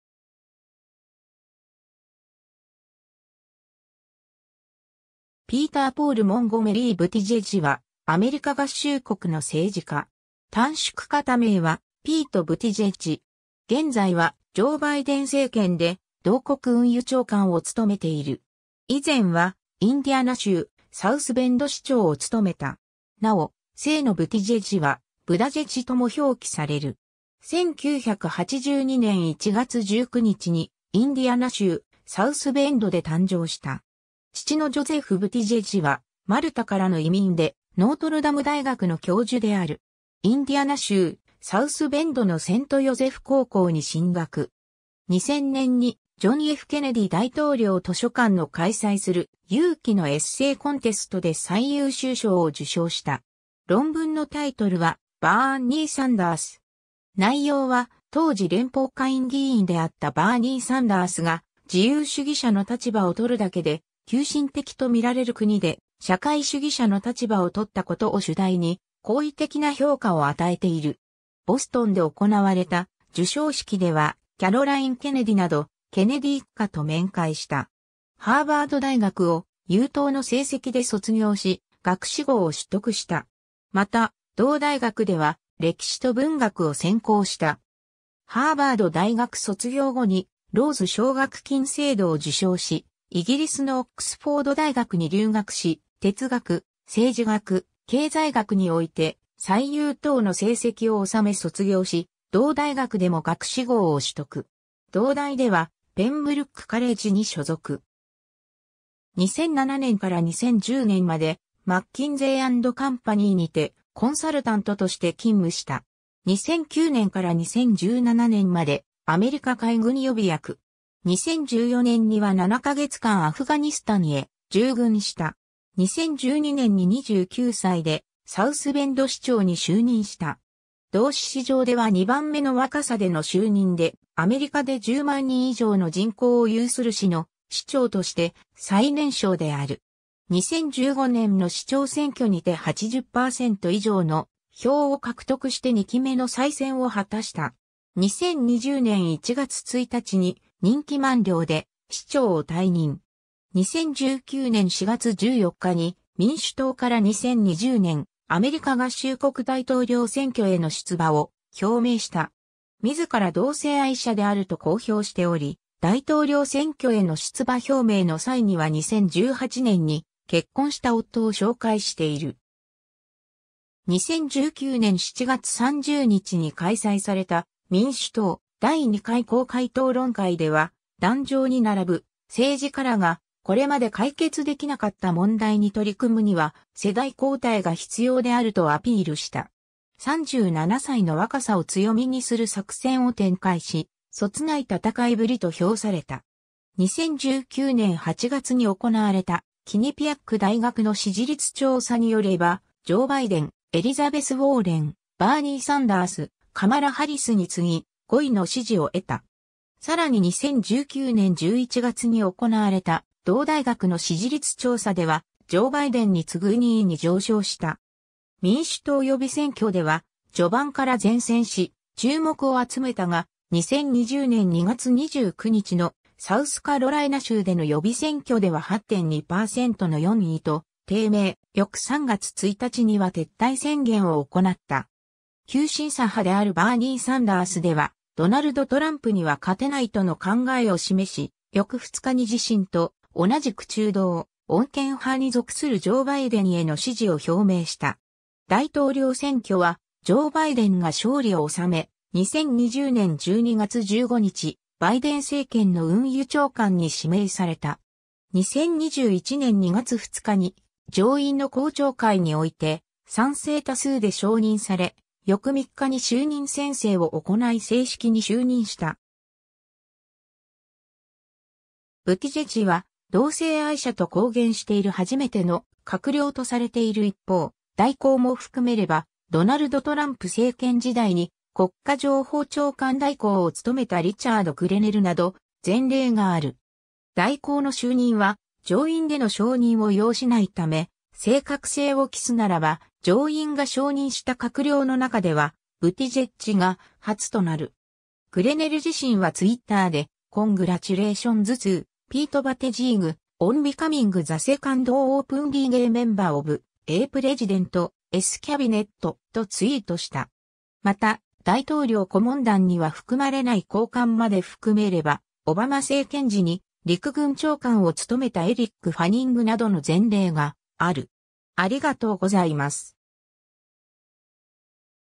ピート・ブティジェッジ
ピーター・ポール・モンゴメリー・ブティジェッジ（英語：Peter Paul Montgomery Buttigieg、[ˈbuːtədʒɛdʒ] BOOT-ə-jej、1982年1月19日 - ）は、アメリカ合衆国の政治家。
短縮形名は、ピート・ブティジェッジ（Pete Buttigieg）。
現在はジョー・バイデン政権で同国運輸長官を務めている。
以前はインディアナ州サウスベンド市長を務めた。
なお姓のブティジェッジは、ブダジェッジとも表記される。
 1982年1月19日にインディアナ州サウスベンドで誕生した。
父のジョゼフ・ブティジェッジはマルタからの移民でノートルダム大学の教授である。
 インディアナ州サウスベンドのセント・ヨゼフ高校に進学。
2000年にジョン・F・ケネディ大統領図書館の開催する「勇気のエッセイコンテスト（Courage Essay Contest）」で最優秀賞を受賞した。
論文のタイトルは「Bernie Sanders」。
内容は当時連邦下院議員であったバーニー・サンダースが「自由主義者の立場を取るだけで急進的と見られる国で社会主義者の立場を取ったこと」を主題に、好意的な評価を与えている。
ボストンで行われた授賞式では、キャロライン・ケネディなどケネディ一家と面会した。
 ハーバード大学を優等（Magna Cum Laude）の成績で卒業し、学士号を取得した。
また、同大学では歴史と文学を専攻した。
 ハーバード大学卒業後にローズ奨学金制度を受賞しイギリスのオックスフォード大学に留学し、哲学・政治学・経済学において最優等（First-Class Honours）の成績を収め卒業し、同大学でも学士号を取得。
同大ではペンブルックカレッジに所属。
 2007年から2010年までマッキンゼー・アンド・カンパニーにて、コンサルタントとして勤務した。
 2009年から2017年までアメリカ海軍予備役。
2014年には7ヶ月間アフガニスタンへ従軍した。
 2012年に29歳でサウスベンド市長に就任した。
同市史上では2番目の若さでの就任で、アメリカで10万人以上の人口を有する市の市長として最年少である。
 2015年の市長選挙にて80パーセント以上の票を獲得して2期目の再選を果たした。
2020年1月1日に任期満了で市長を退任。
 2019年4月14日に民主党から2020年アメリカ合衆国大統領選挙への出馬を表明した。
自ら同性愛者であると公表しており、大統領選挙への出馬表明の際には2018年に結婚した夫を紹介している。
 2019年7月30日に開催された民主党第2回公開討論会では、壇上に並ぶ政治家らがこれまで解決できなかった問題に取り組むには世代交代が必要であるとアピールした。
37歳の若さを強みにする作戦を展開し、そつない戦いぶりと評された。
 2019年8月に行われたキニピアック大学の支持率調査によれば、ジョー・バイデン(32パーセント)、エリザベス・ウォーレン(19パーセント)、バーニー・サンダース(15パーセント)、カマラ・ハリス(7パーセント)に次ぎ、5位（5パーセント）の支持を得た。
さらに2019年11月に行われた同大学の支持率調査では、ジョー・バイデンに次ぐ2位に上昇した。
 民主党予備選挙では序盤から善戦し注目を集めたが、2020年2月29日のサウスカロライナ州での予備選挙では8.2パーセントの4位と低迷、翌3月1日には撤退宣言を行った。
急進左派であるバーニー・サンダースではドナルド・トランプには勝てないとの考えを示し、翌2日に自身と同じく中道・穏健派に属するジョー・バイデンへの支持を表明した。
 大統領選挙はジョー・バイデンが勝利を収め、2020年12月15日、バイデン政権の運輸長官に指名された。
2021年2月2日に上院の公聴会において賛成多数で承認され、翌3日に就任宣誓を行い正式に就任した。
 ブティジェッジは同性愛者と公言している初めての閣僚とされている一方、代行も含めればドナルド・トランプ政権時代に国家情報長官代行を務めたリチャード・グレネルなど前例がある。
代行の就任は上院での承認を要しないため、正確性を期すならば、上院が承認した閣...